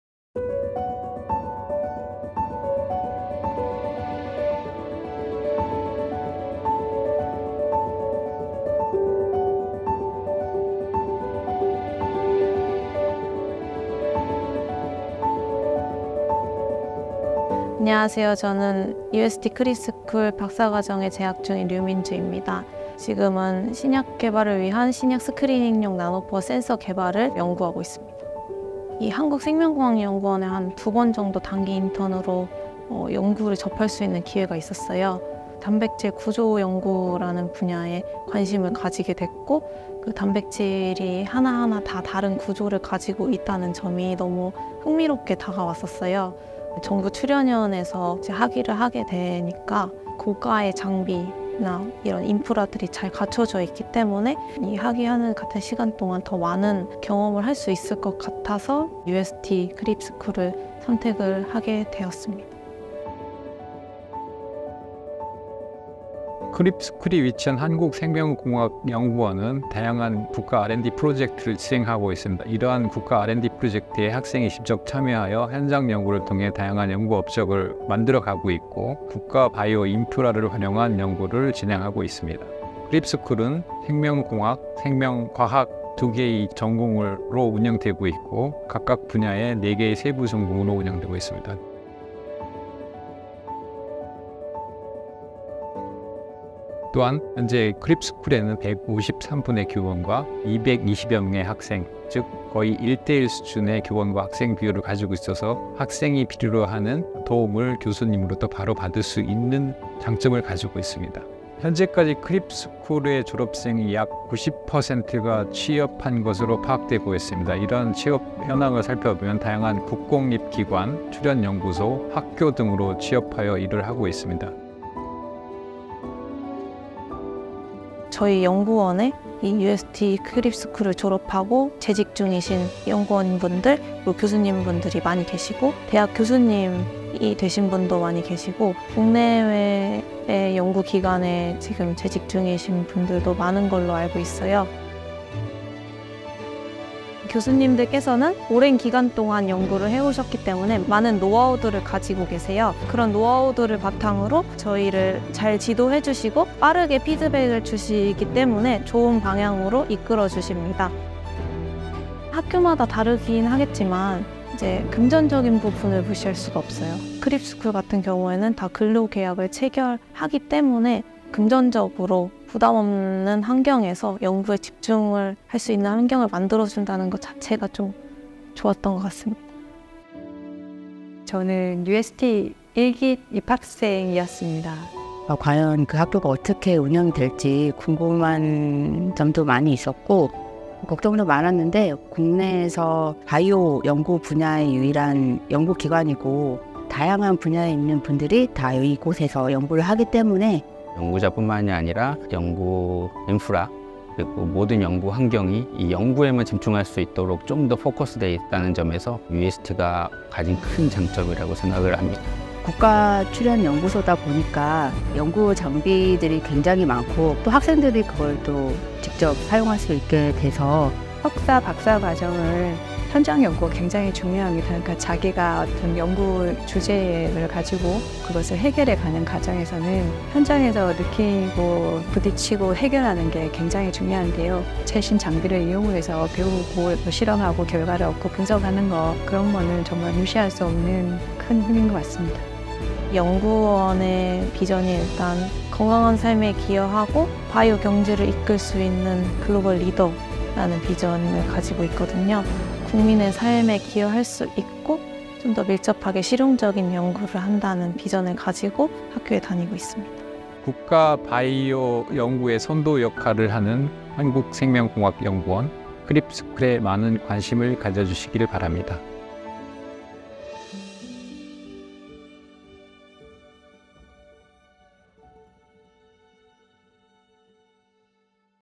[UST KRIBB 브랜드필름] 한국생명공학연구원에서 직접 육성하는 학생들
진화, 유전, DNA... 몹시 흥미로운 소재죠?
정부출연연구기관인 한국생명공학연구원(KRIBB)에서 활발하게 연구 중인 것들인데요.

혹시 한국생명공학연구원에 소속된 학생들이 있다는 사실, 알고 계신가요?
UST KRIBB 스쿨 학생들과 교수, 졸업생이 UST KRIBB 스쿨에 대해 얘기합니다.

학부 졸업 후 대학원을 알아보시는 분들,
대학원을 졸업 후 박사 과정을 알아보시는 분들,
국가에서 운영하는 연구소 한국생명공학연구원에서
과연 어떻게 학생들을 육성하고 지원하는지 확인 해보세요!